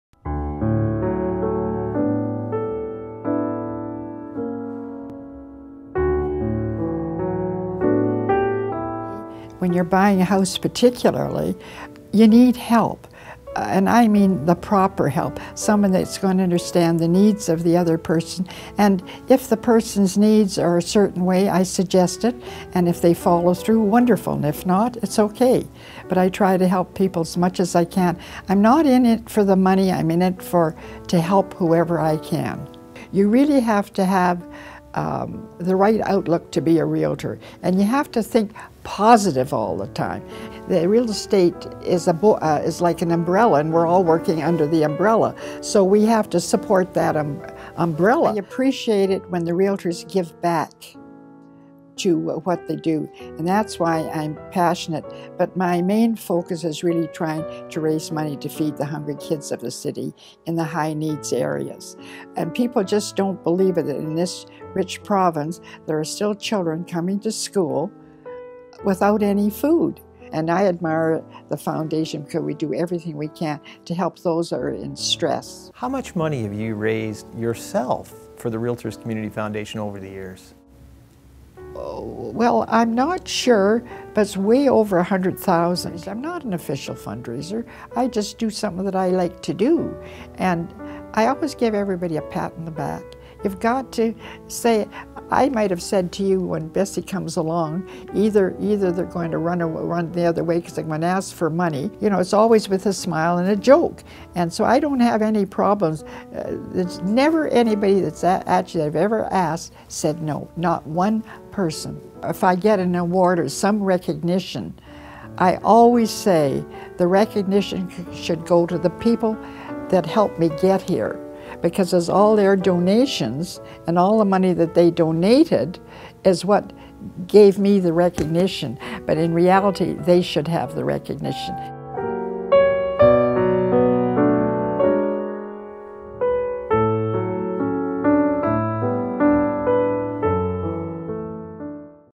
REALTORS® Community Foundation - Volunteer of Distinction Award - Bessie Goldstick
REALTORS® Community Foundation - Volunteer of Distinction Award. The Volunteer of Distinction Award is presented to a REALTOR® who has gone above and beyond expectations and proven to be a tireless, hardworking supporter and promoter of the REALTORS® Community Foundation giving of their time, their talent, and their heart.